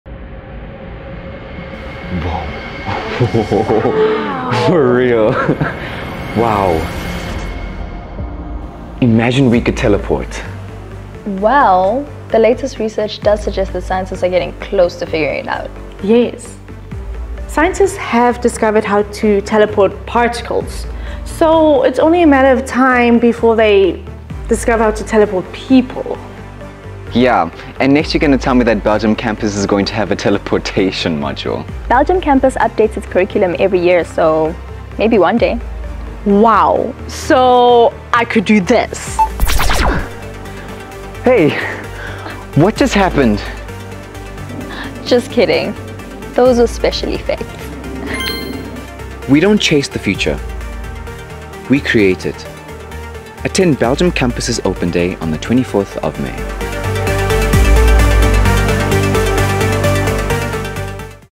Attend our 24 May Open Day
Teleportation was fiction. Now it's cutting-edge research! Attend our Open Day to see how you can study the latest tech!